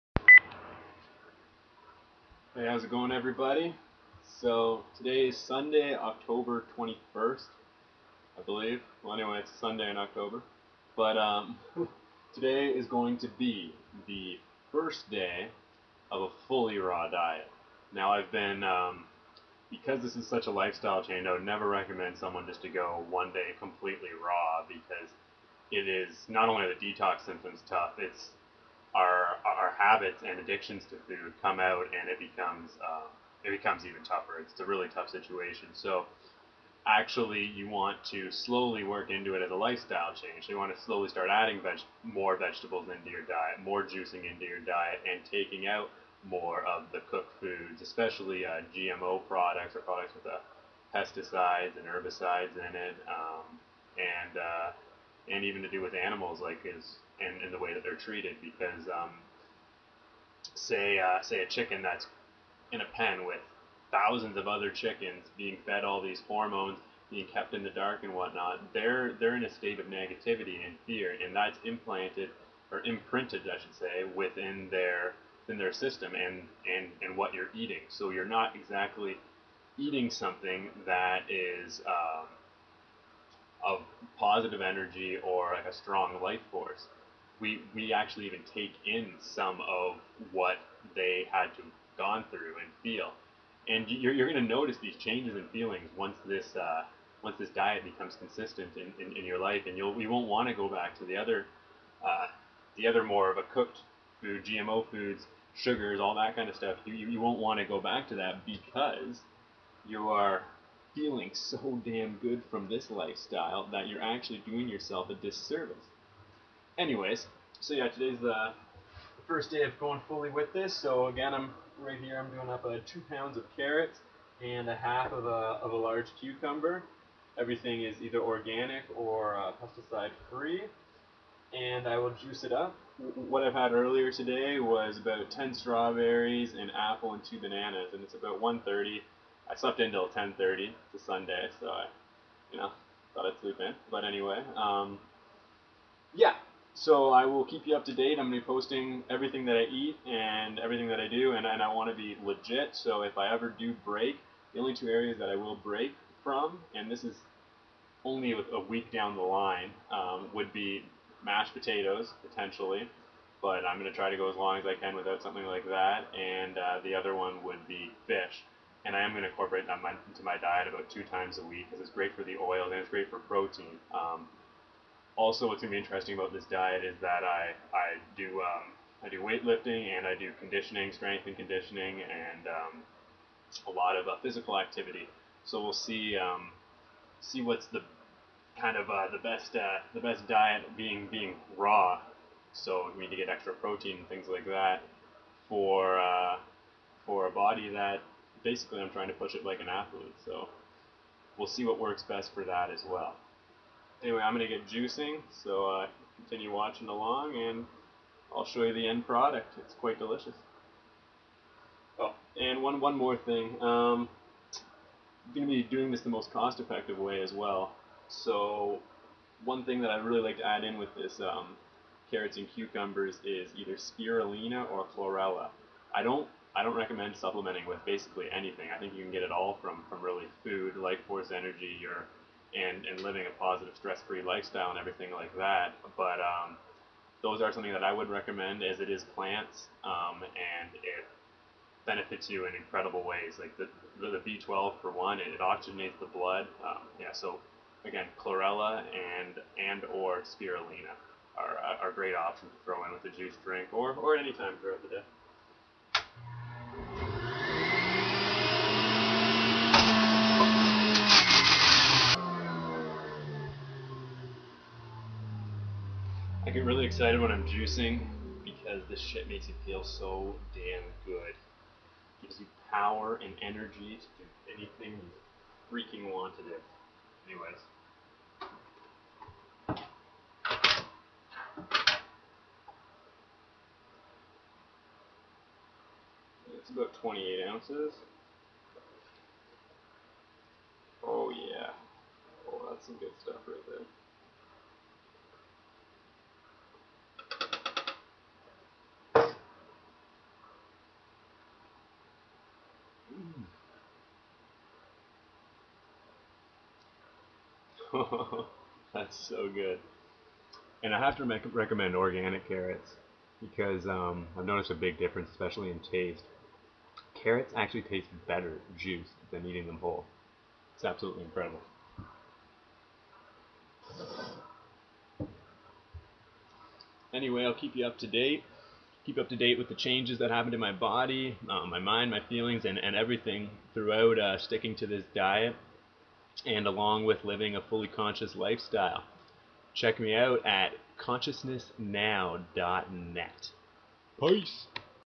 Day 1 Fully Raw Diet - Delicious Juice Recipe
I have been progressing towards my second effort of a fully raw diet. I had very serious chronic digestive problems that have been getting better since changing my diet, bringing down stress, becoming more conscious etc. I am doing this diet in order to fully alleviate any digestive issues and bring myself into a higher state of well being. I have already noticed dramatic results in my feeling state from sticking to a raw mostly organic diet. I will eat raw & cooked wild fish sometimes and if things get really difficult I may eat some mash potatoes, which I will document, The recipe in the video is simply 2 lbs of organic carrots, 1/2 a pesticide free cucumber (full cucumber works as well). This recipe is delicious and I am only going to share recipes that taste good or at least decent, because this is a lifestyle change so even though it being healthy is good enough reason to eat it, ya want to make it taste good so you stick with it I have found. I also usually put in 5 - 10 grams of powdered Spirulina. Chlorella also works great. They both can clean your blood by oxygenizing it, clear your mind with the b12 and promote well-being. Well-being is all about living in a good feeling state. The raw lifestyle will improve your well-being so drastically you will not want to go back. However at first the detox symptoms will be tough, as we are all full of toxins, get support and make this a lifestyle change by slowly working in to it. I can assure you it is worth it!! Much Love, from C-Dub!